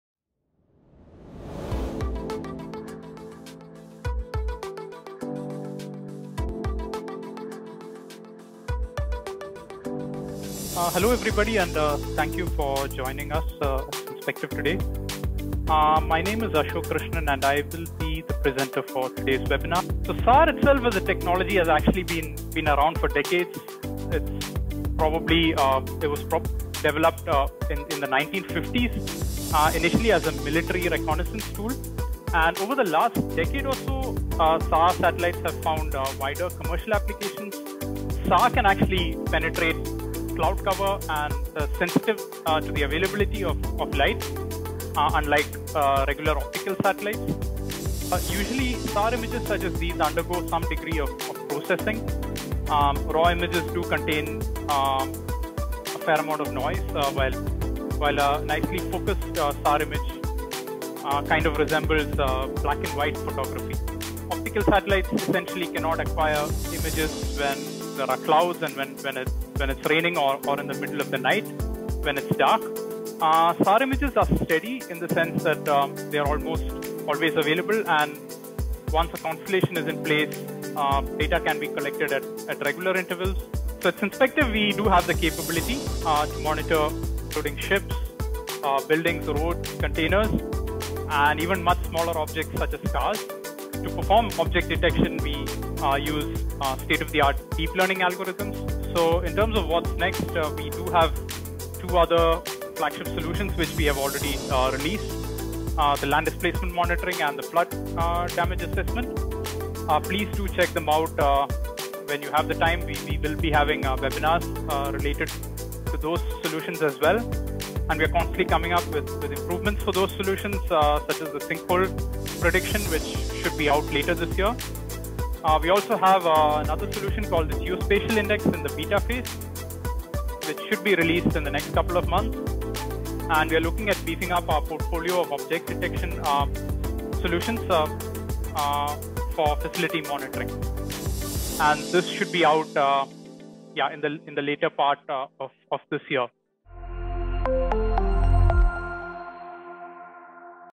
Synspective Webinar Series - "Machine Learning based Object Detection using SAR data" digest movie
This is a digest of a webinar held by Synspective.

About the webinar.
Business decisions have become increasingly data-driven over this decade. As a result, companies need to be aware of their competitors’ activities and the general market conditions to make informed business decisions.

The monitoring of various facilities in the areas of interest, even owned by competitors, becomes essential apart from significant hubs of economic activity. However, many of these facilities are in areas that are difficult to monitor physically at regular intervals of time. Furthermore, the emergence of the Covid-19 pandemic over the last year and its associated movement restrictions has made facility monitoring a very challenging exercise.

The demand for remote monitoring solutions has increased, especially the attractiveness of satellite-based solutions. SAR (*) satellites, which operate in the low Earth orbit, hold a lot of promise in the context of remote facility monitoring as they are capable of operating day and night under any weather condition. Object detection lies at the heart of any satellite-based facility monitoring platform.

This webinar introduced our state-of-the-art machine learning-based object detection technology that utilizes data obtained from SAR satellites. Some exemplar object detection case studies and their potential applications in the context of remote facility monitoring will also be presented during this webinar.

*Synthetic Aperture Radar (SAR) is a form of radar that is used to create two-dimensional images or three-dimensional reconstructions of objects, such as landscapes. SAR satellites send microwaves to Earth and receive the reflected signal to create an image of the target area.

Speaker   Ashok Krishnan
Customer Solutions Engineer @Synspective Inc.

After completing his undergraduate education in India, Ashok received the PhD degree in electrical and electronic engineering from Singapore’s Nanyang Technological University in 2019. He has about 9 years of research and industry experience related to electrical power and energy systems. Earlier this year, he transitioned to a career in the satellite technology industry. At Synspective, Ashok works on translating client requirements to workable solutions by bridging the business development and technical teams. Ashok is keen to explore how satellite data in general and SAR data in particular can help users in diverse industries make data-driven business decisions. In his spare time, Ashok enjoys outdoor activities such as running and hiking.

About us.